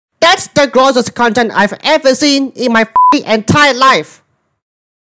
My Opinion on An1-One's Content
That's right! An1-One is the grossest user for making repeated body invasion videos on her content! It's completely grossed-out and cringed! I hope @BlueThunderAndy and @TylerSkovdalStudio can agree with my video! Thank god she closed her account!

Video inspired by @BlueThunderAndy/@BlueThunderAndy2ndChannel

Note: This is Mina from Jelly Jamm while she's angry. I hope @SoniaPandaArtist should see this.

and all the HCSBH6969 logos